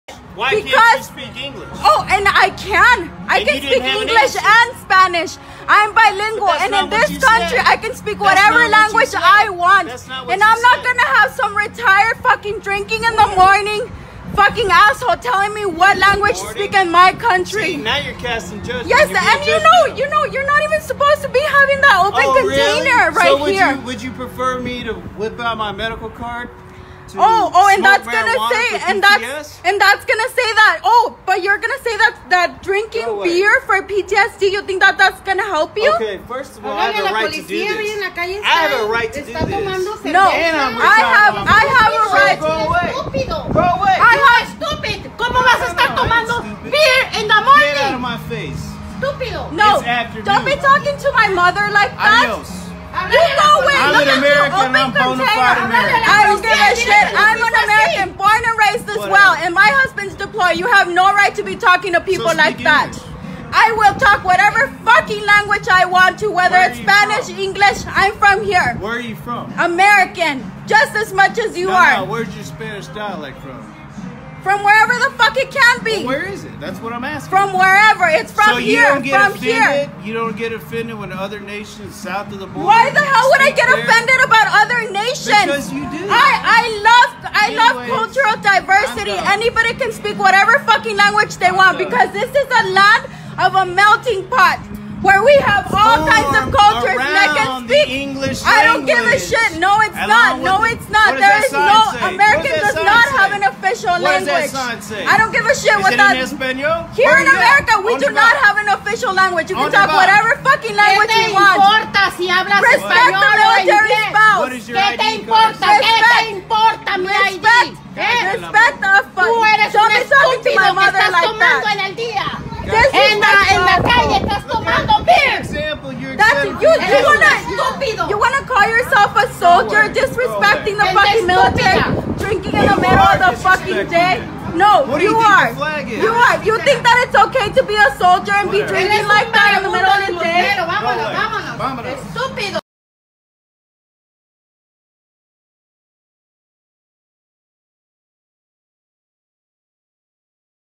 'This is America': Man tells woman to speak English at Cracker Barrel, she refuses to back down
Business
Yahoo Lifestyle  Hope Schreiber,Yahoo Lifestyle 8 hours ago
Reactions  Reblog on Tumblr  Share  Tweet  Email

A white man told a woman, who was speaking Spanish with her mother at a Cracker Barrel, to speak English as “This is America.” She captured the incident on video. (Photo: Facebook)
A video, which depicts a woman confronting a man who told her to speak English outside of a Cracker Barrel in El Paso, Texas, is making the rounds on social media thanks to the woman’s impassioned response.

Jasmin Nolan and her mother were having a conversation in Spanish outside the restaurant when a man told her, “This is America. Speak English.”

Nolan began filming the incident after the comment and the man can be heard asking, “Why can’t you speak English?”

“Oh I can — I can speak English and Spanish,” she says in the video. “I’m bilingual and in this country, I can speak whatever language I want.”

The woman also points out that the man is holding an open can of beer outside. The man defends his open container by explaining he is a veteran suffering from PTSD, while Nolan explains her husband is currently serving.

The video of the confrontation lasts nearly two and a half minutes.